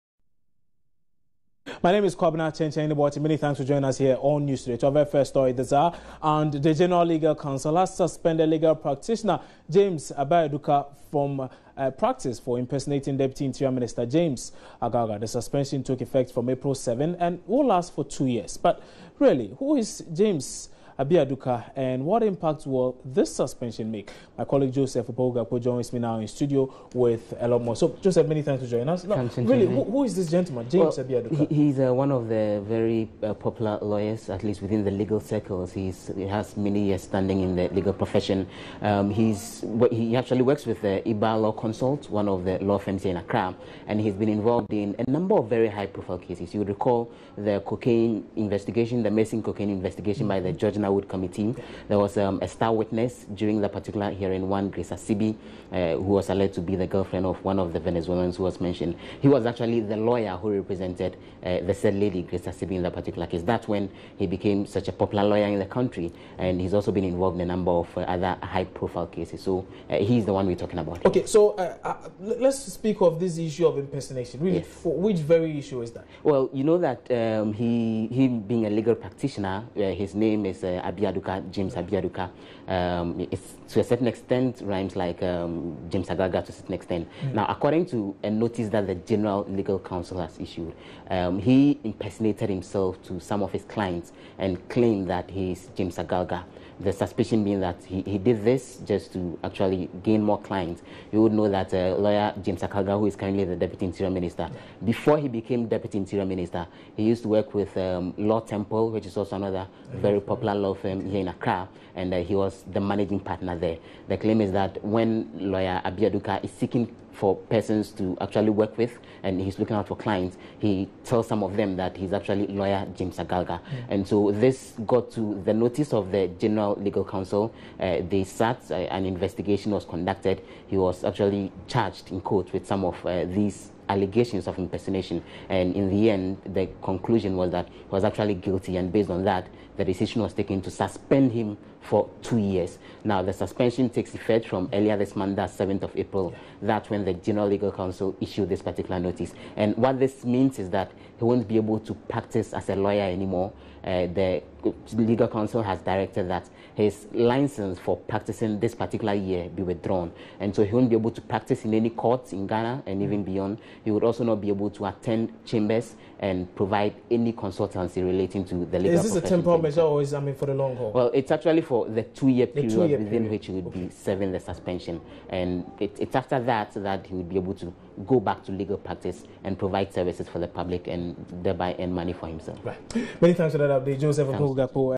Lawyer suspend
Joseph Opoku Gakpo speaks to Kwabena Kyenkyenhene Boateng on lawyer's suspension